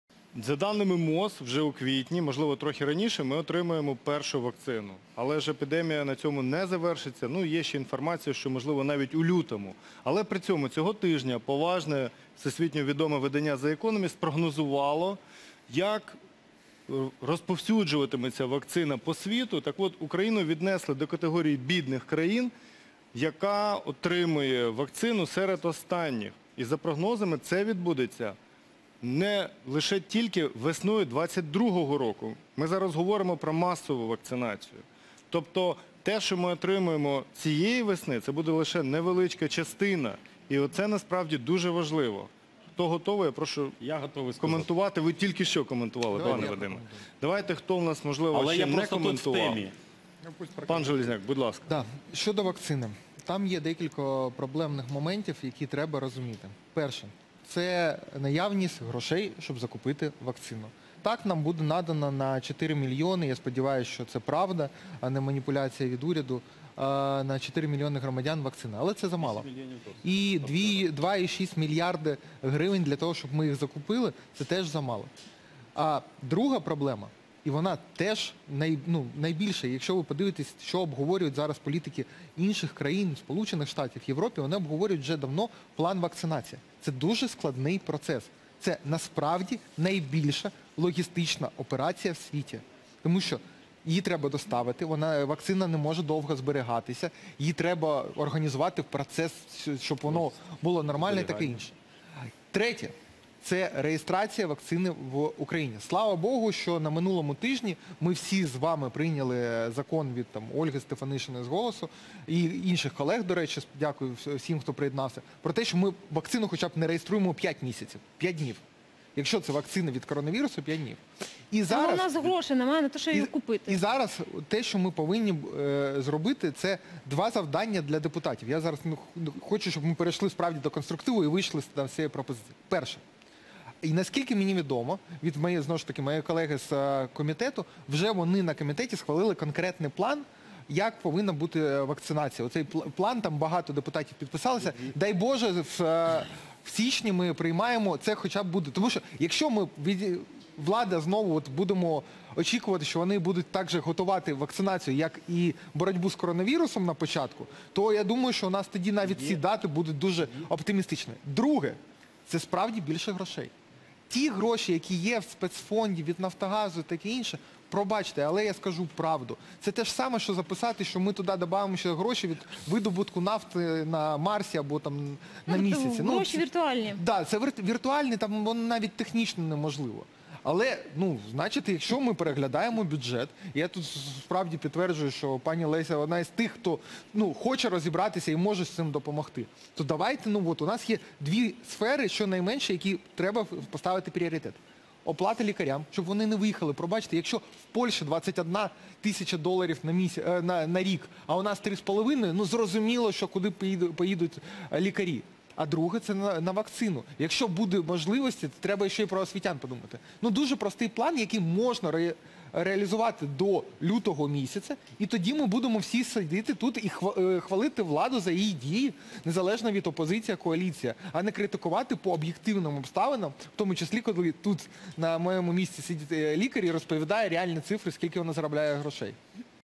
"Це найбільша логістична операція у світі": депутат розповів про вакцинацію від коронавірусу
На минулому тижні був прийнятий закон про те, що вакцину від коронавірусу Україна не реєструє принаймні п'ять днів